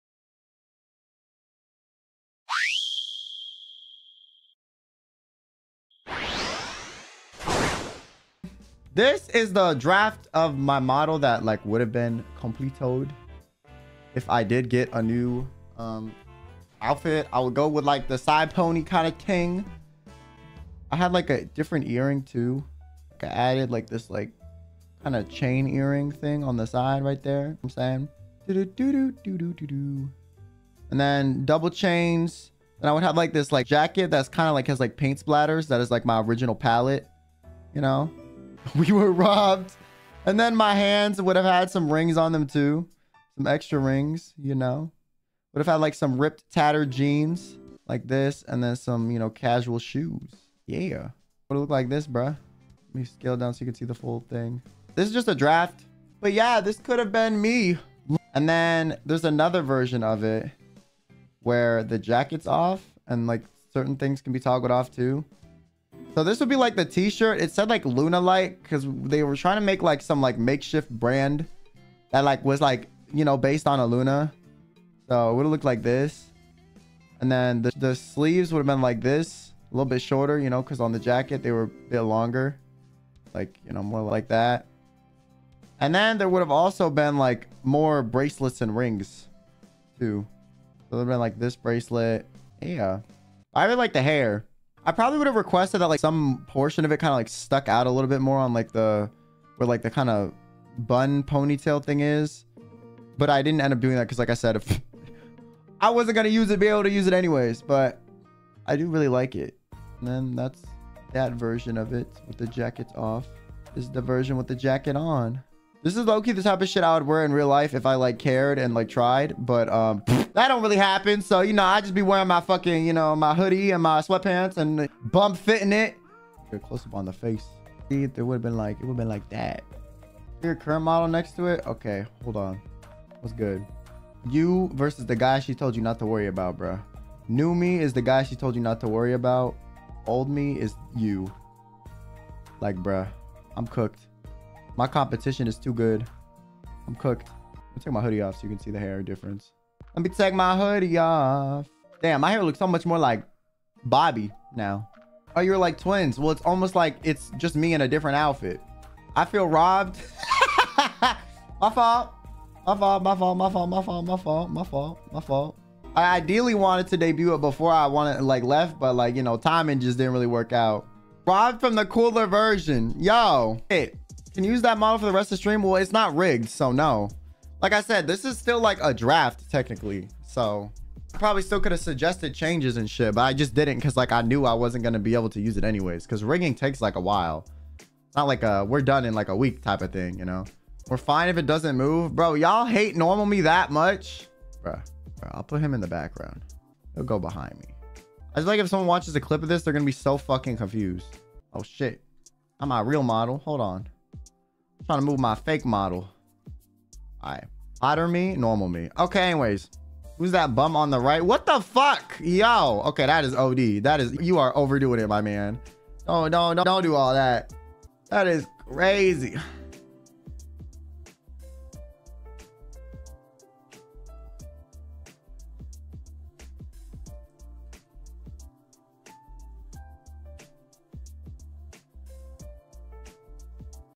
Kyo Showed How His New Outfit Would Be If He Didn't Graduate
Clip title :  - Nijisanji EN Clips
Stream title : LAST ZATSU UEUEUEUE